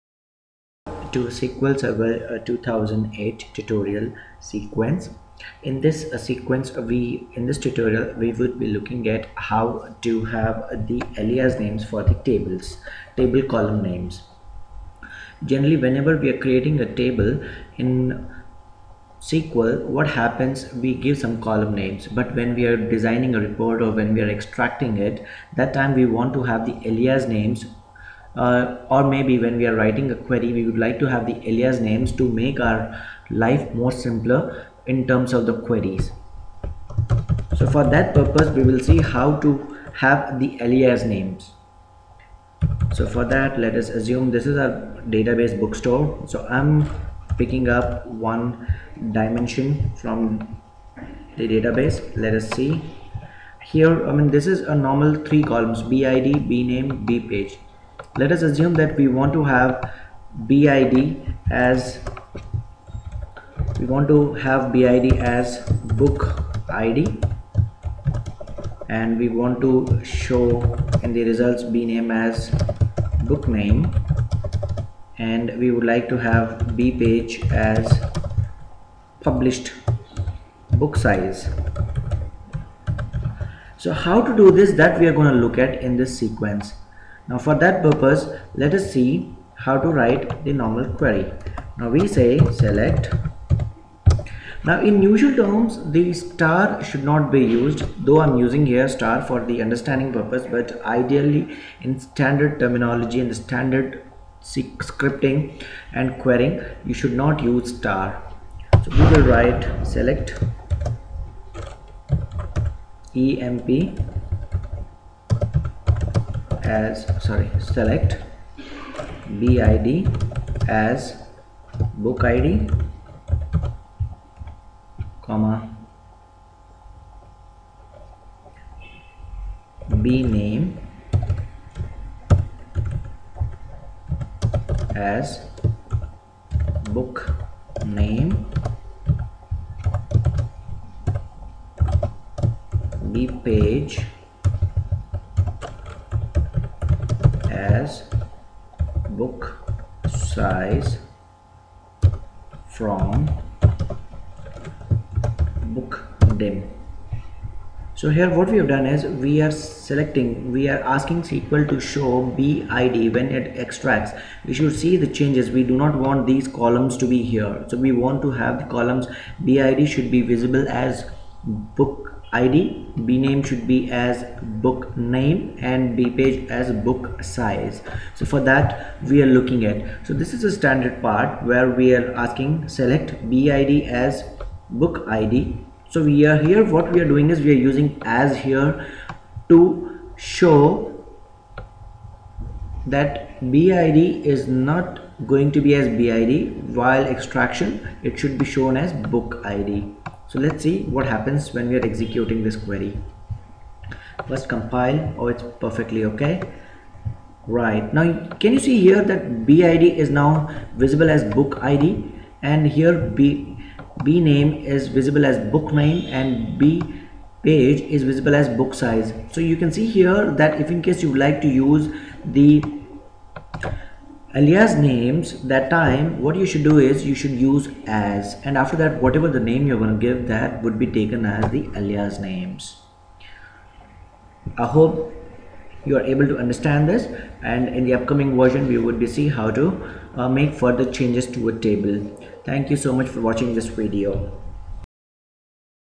Microsoft SQL Server Tutorial for the Beginners- How to get Table column alias names
This video tells us how to use the alias names in the SQL server.Alias names are the pseudo names of the column to make the undertanding  better and faster.